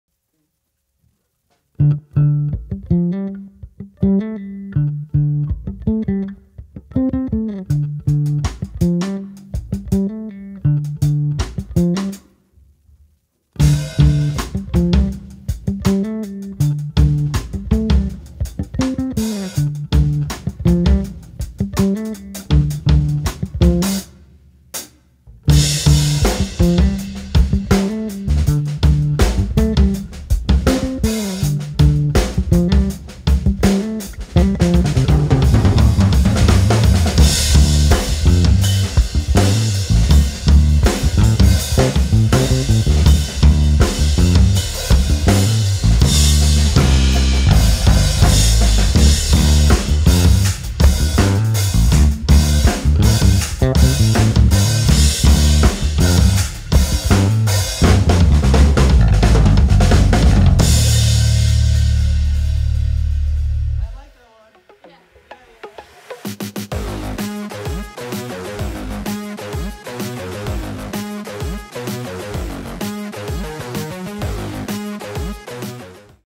SundayGroove #11 (feat. Aubrey Harris)
Hey guys,

Today I sat down with the awesome Aubrey Harris to record SundayGroove no. 11! Aubrey came up with the bassline pretty quickly and we both liked the melodic idea a lot. From that point on all I had to do was to create a fitting beat. The session went very smooth and we had a good amount of fun doing this video. Hope you like it!

All the best, Lars Nijman

P.S.: In two weeks I'm recording some drumcovers again ;)

Istanbul cymbal setup:
14 inch Istanbul Mel Lewis HH
10 inch Alchemy mini China
12 inch Istanbul mini China
18 inch Istanbul Dark crash
21 inch Istanbul Vezir ride